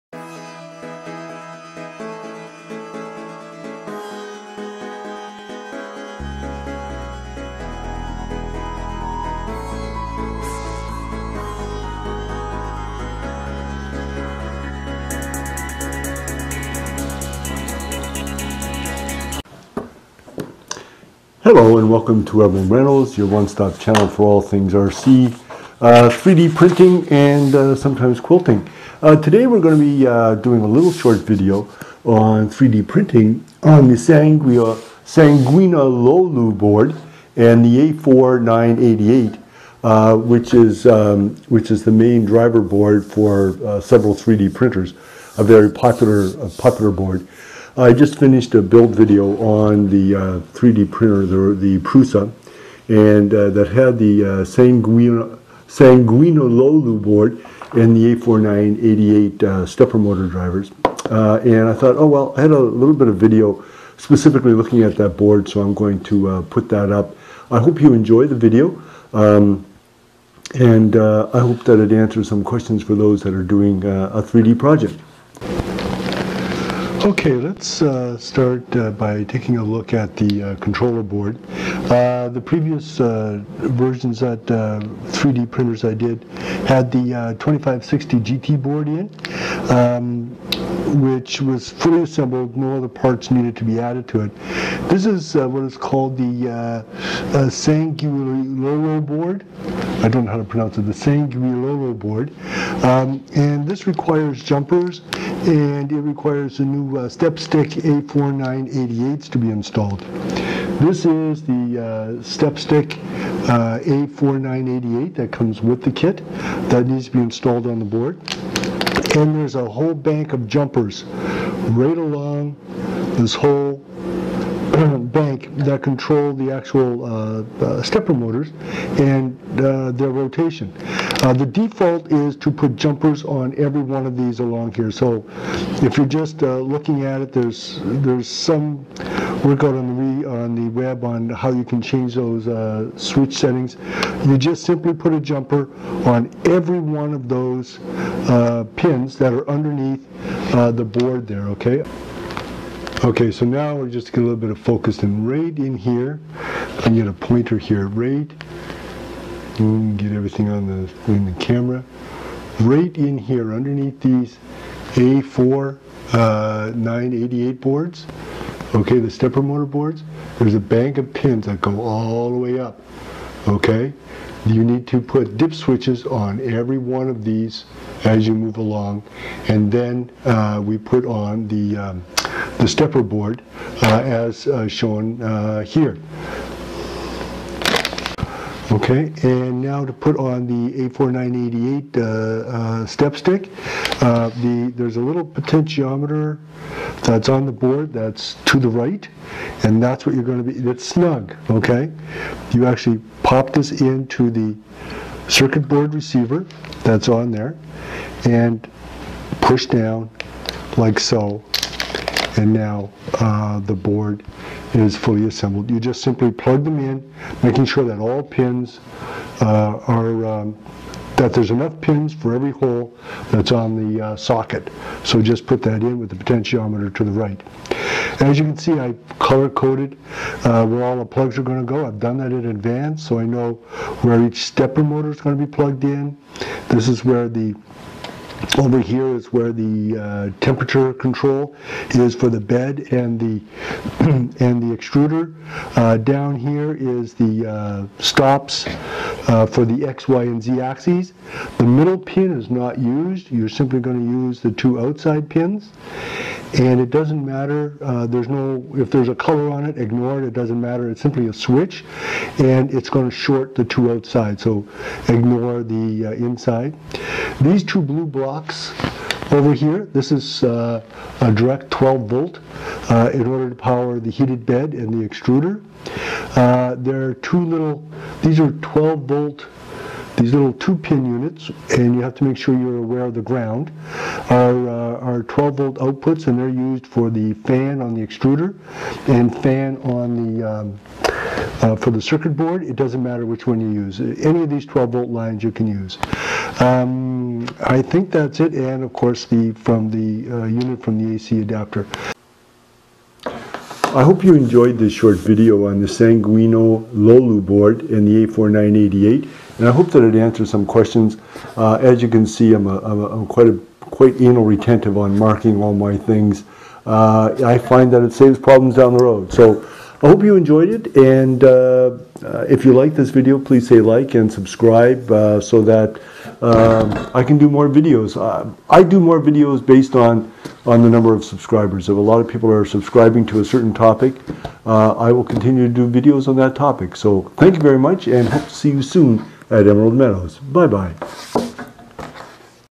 A closer Look at the Sanguinololu 3D Printer Controller
A closer look at the Sanguinololu and the A4988 Stepper SiteStep